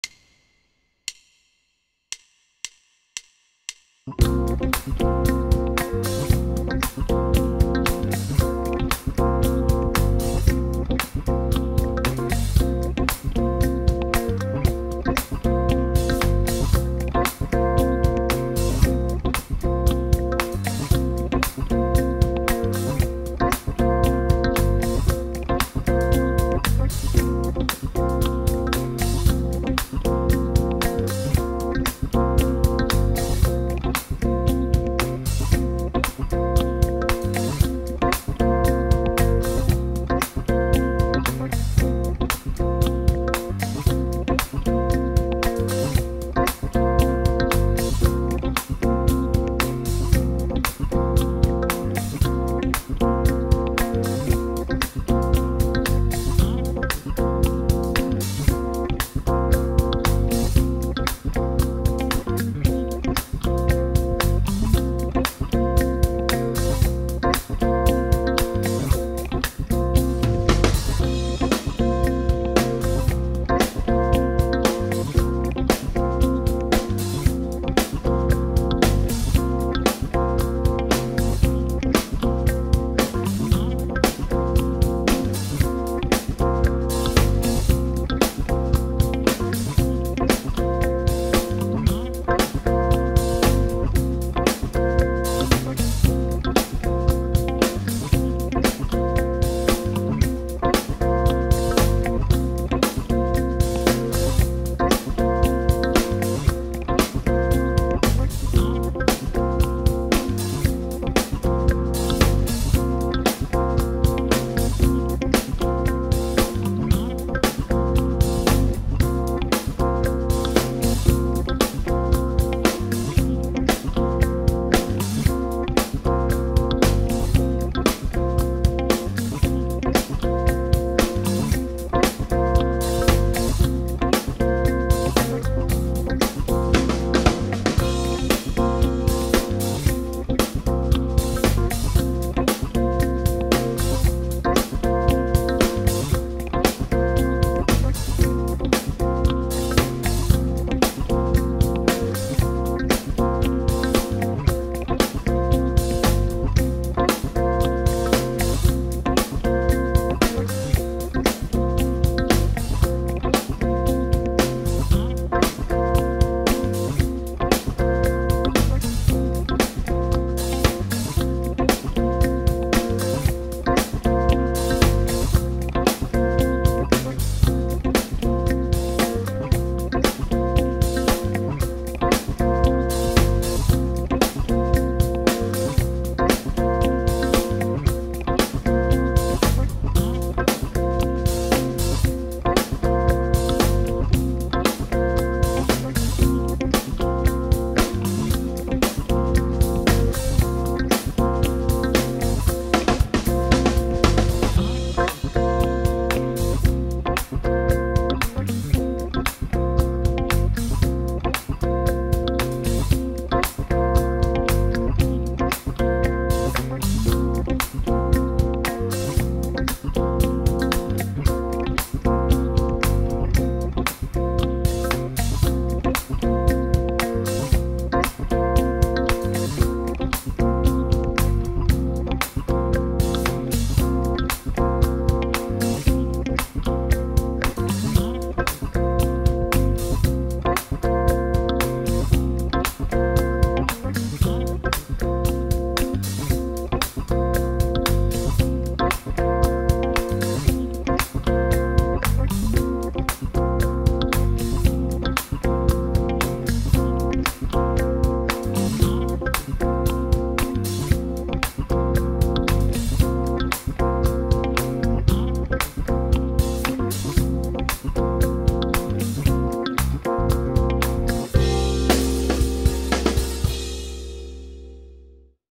BACKING TRACK - C MIXOLYDIAN - Funk - 115 bpm
Here's a BACKING TRACK with a Funk feel to practice the Mixolydian mode !

Great for guitar, horns, piano, etc.

Backing Track for personal use only.